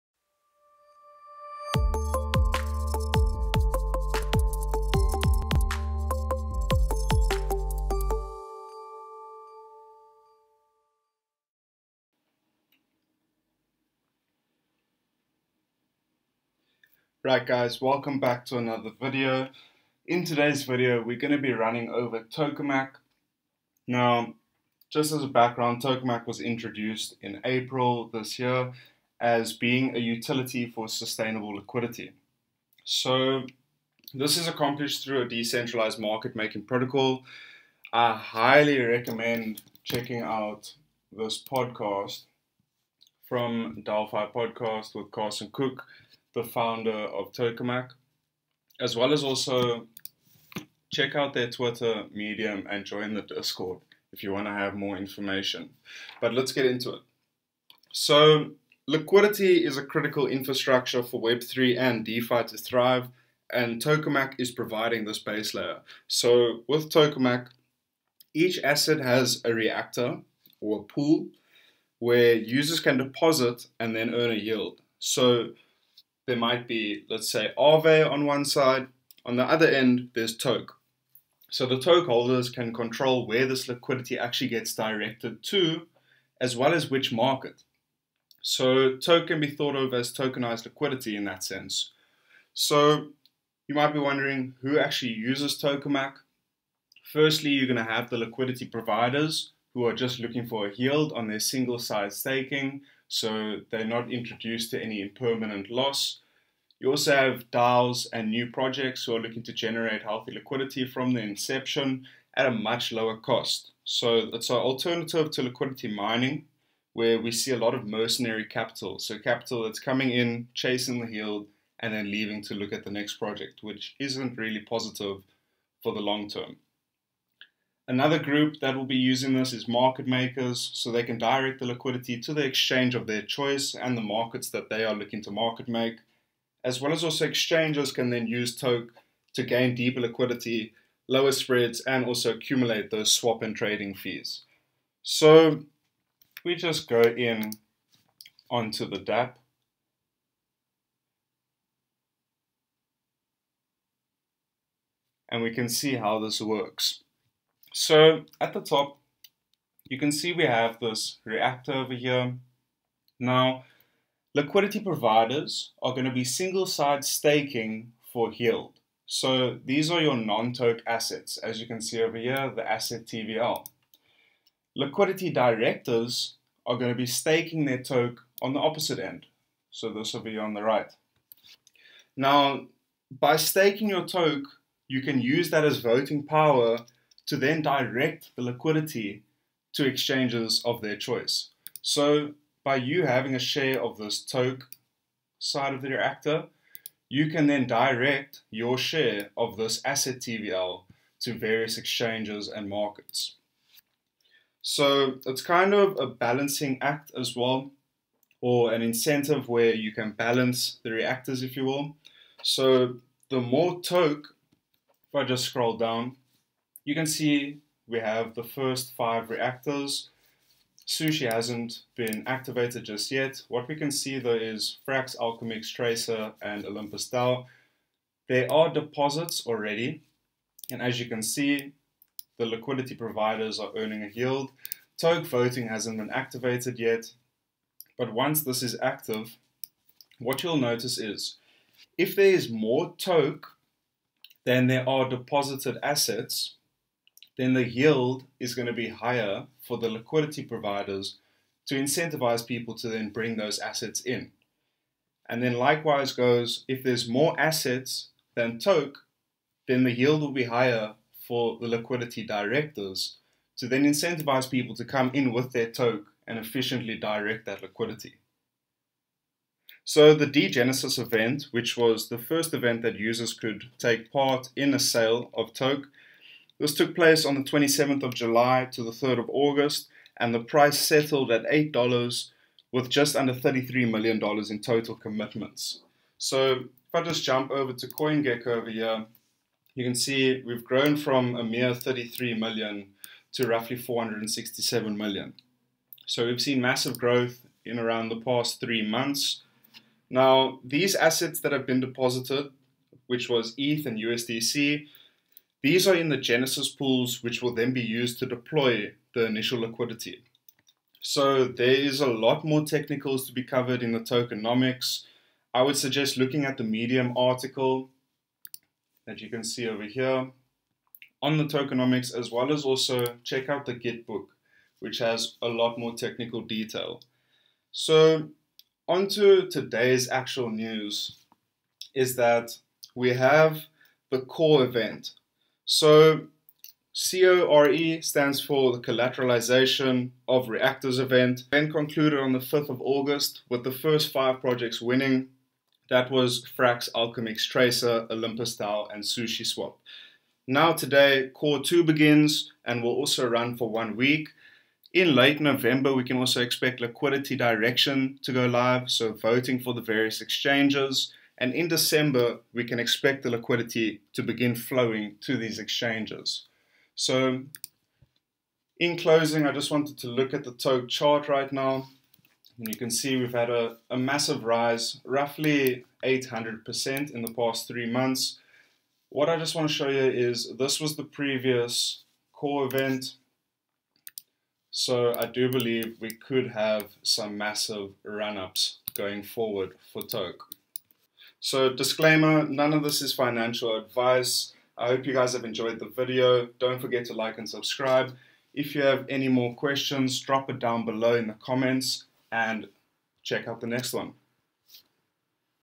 TOKEMAK 10x Gem | C.o.R.E 2 Begins
Tokemak is a decentralized market making protocol designed to create efficient, sustainable liquidity across DeFi. Tokemak's native token, TOKE, serves as tokenized liquidity, enabling stakers to direct the protocol's TVL as liquidity across DeFi markets. The C.o.R.E 2 begins today as 45 candidates battle for the next 5 spots - winning protocols being able to stand up their own reactor. In order to vote for the top 5 reactors, you need TOKE.